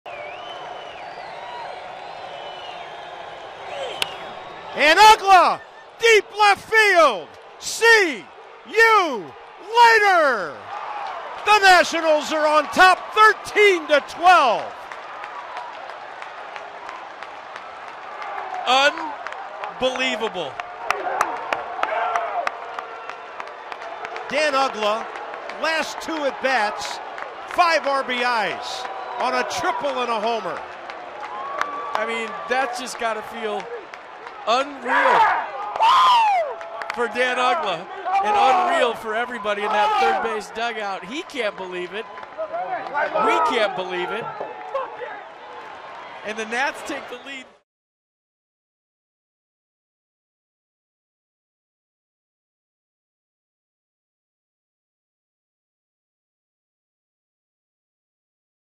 Uggla launches a three-run shot for the lead
4/28/15: Dan Uggla crushes a three-run home run to deep left off Jason Grilli, giving the Nationals a 13-12 lead in the top of the 9th